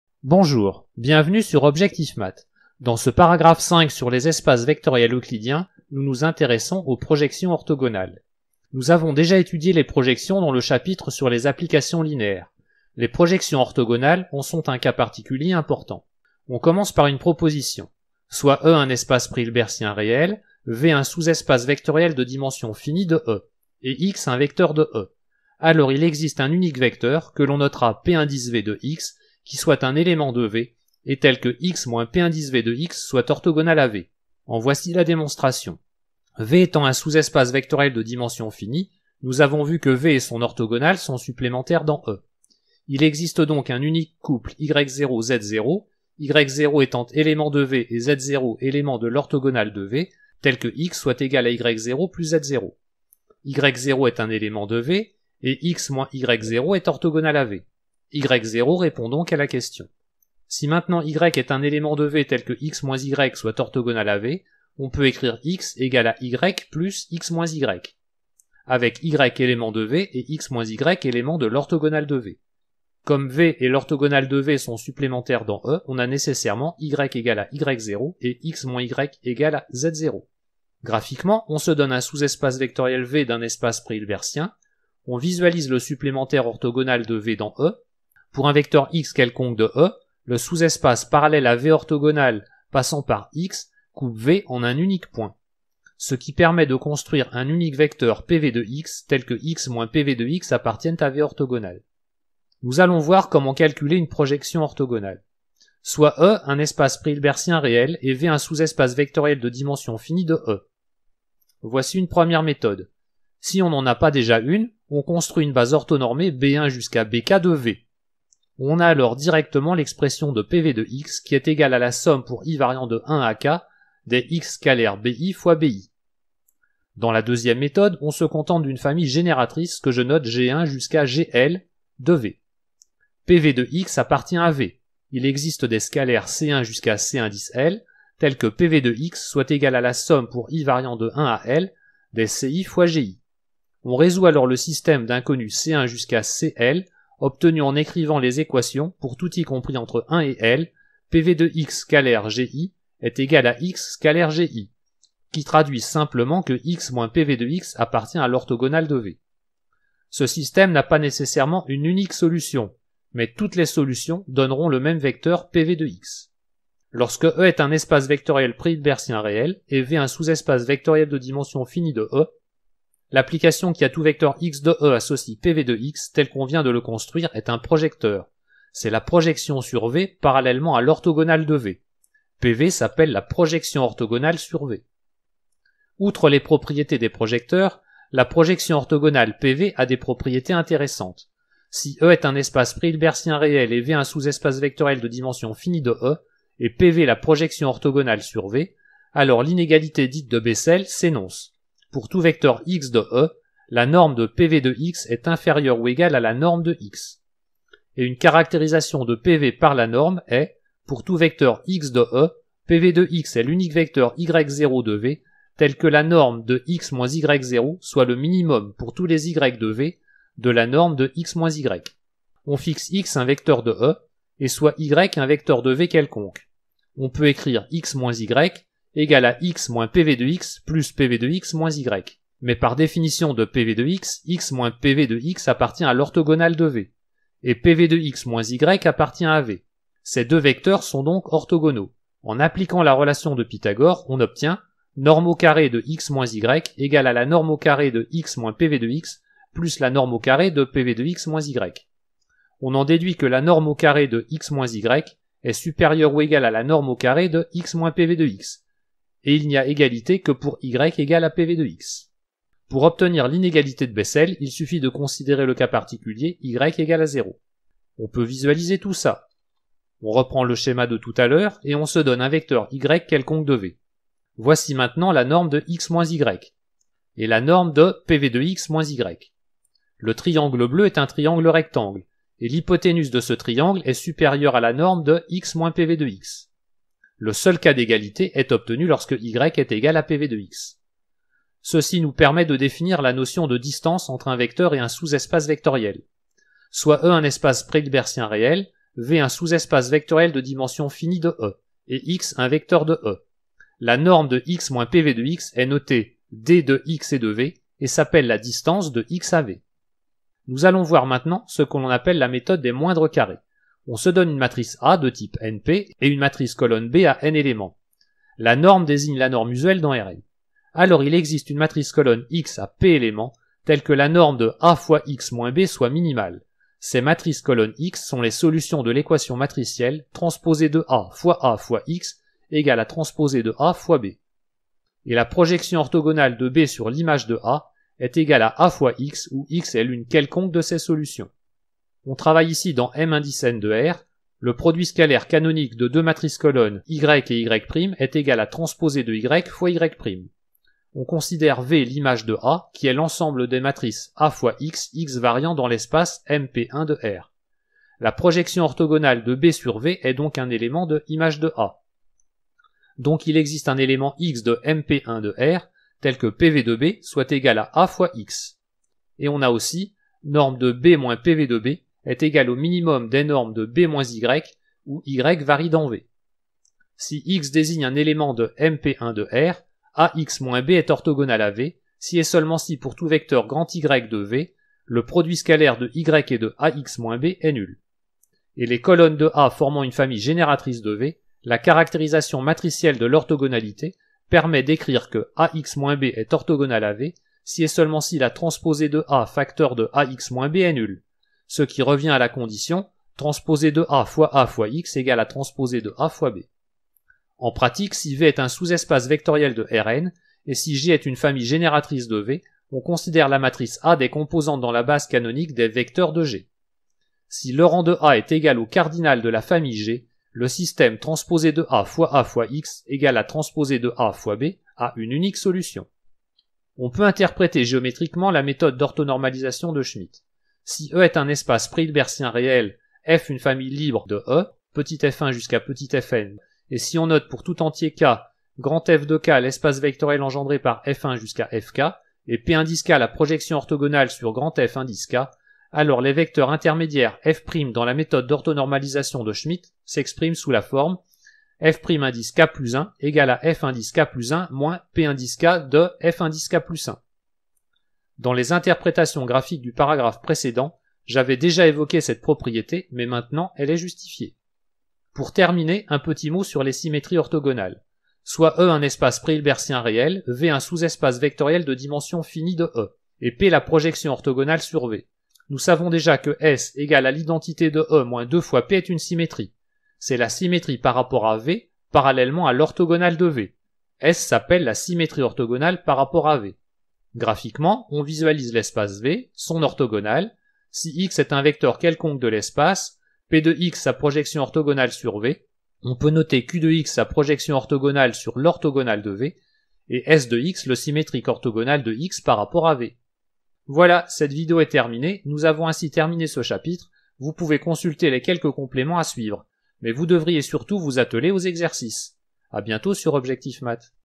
Espaces vectoriels euclidiens §5 Projections orthogonales
BUG :
à 7'55  il faut remplacer s=Id-2p  par s=2p-Id
par contre si q est la symétrie orthogonale par rapport à l'orthogonal de V on a s=id-2q